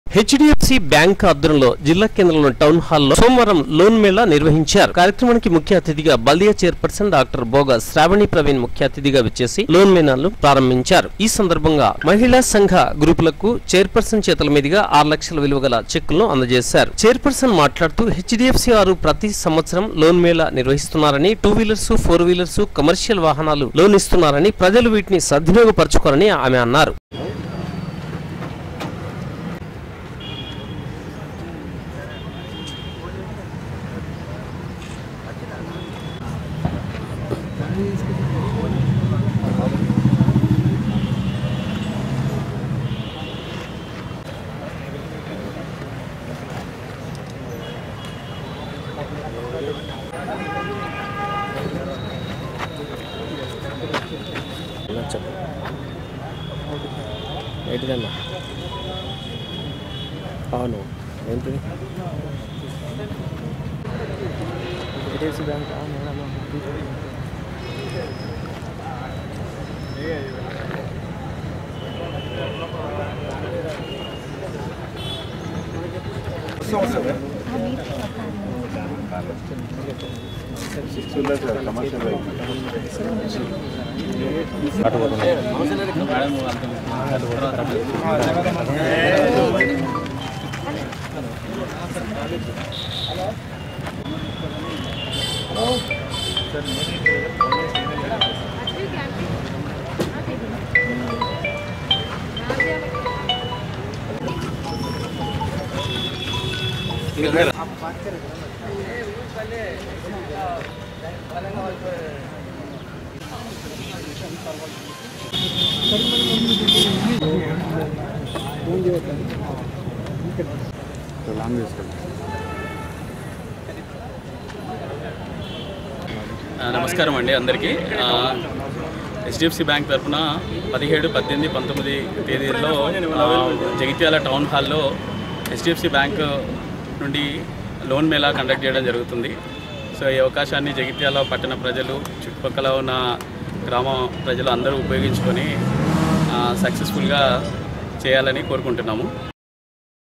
17.02.20| DSC | Jagtial |హెచ్ డి ఎఫ్ సి వారి లోన్ మేళ ప్రారంభించిన బల్దియా చైర్ పర్సన్
| Durga Siti Cable | News |హెచ్ డి ఎఫ్ సి వారి లోన్ మేళ ప్రారంభించిన బల్దియా చైర్ పర్సన్